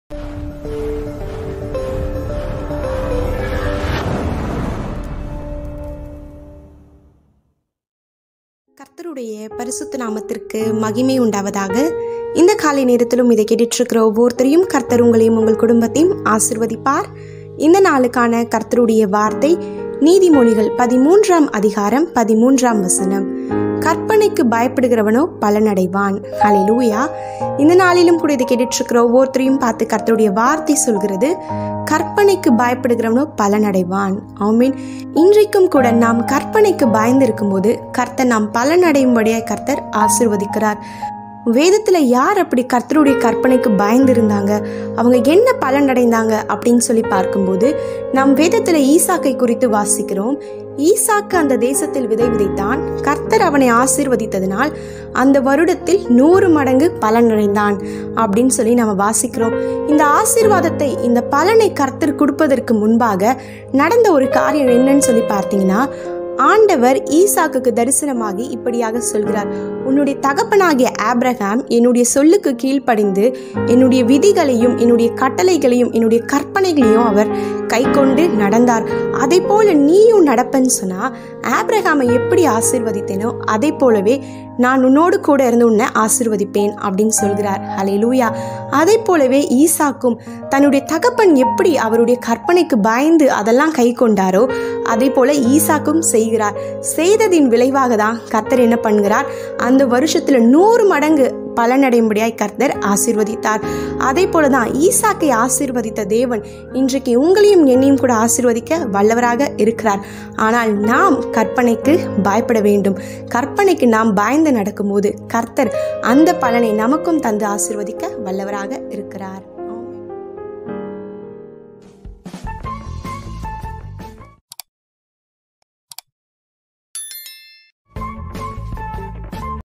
TODAY'S VERSE | PROVERBS 13:13 | JULY 10 2022
Today's Verse

He that feareth the commandment shall be rewarded.
Proverbs 13:13

இன்றைய வார்த்தை

கற்பனைக்கு பயப்படுகிறவனோ பலனடைவான்.
நீதிமொழிகள் 13:13

God Will Bless You According To The Verse

ALSO WATCH